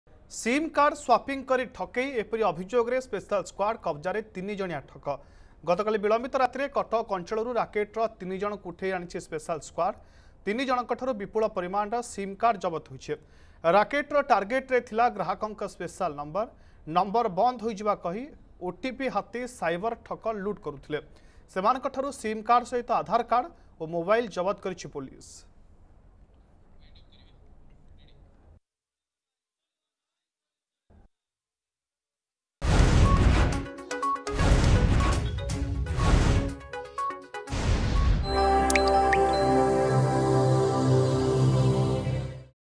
SIM Card Swapping Racket | ArgusNews
Cheating by swapping SIM cards. In such a case, there are 3 known fraudsters in the possession of the Special Squad. Special Squad picked up 3 rackets from Cuttack area late last night.